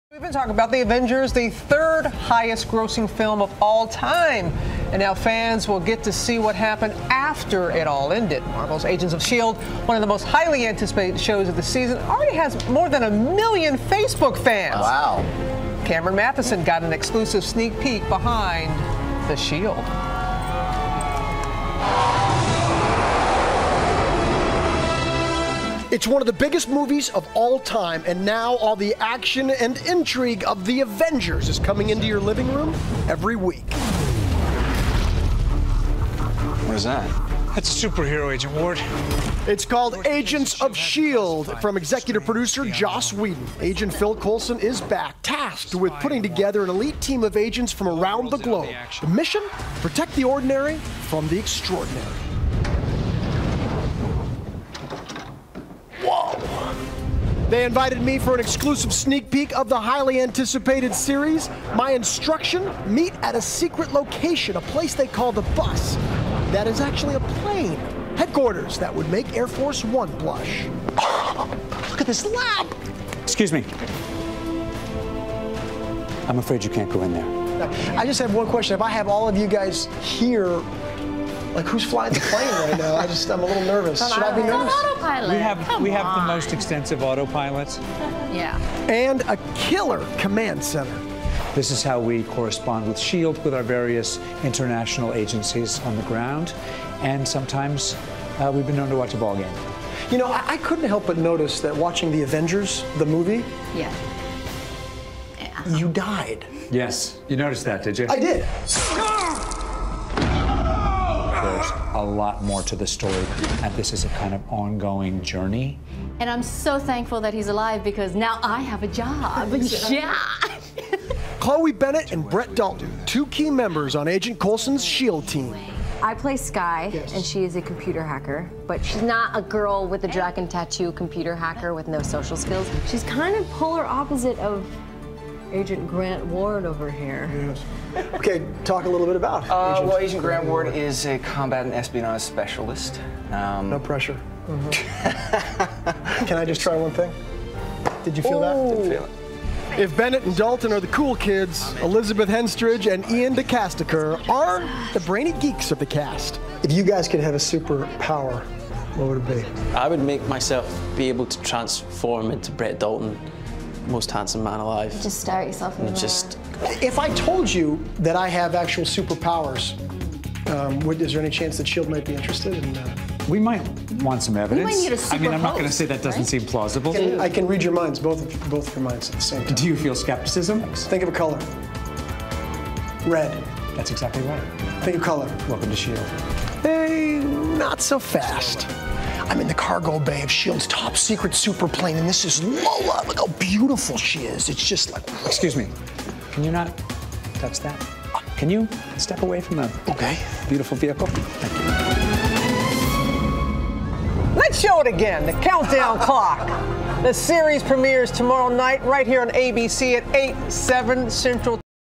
Marvel`s `Agents of SHIELD` Continues `The Avengers` Stor
ABC News' Cameron Mathison gives you an inside look at one of the year's most anticipated shows.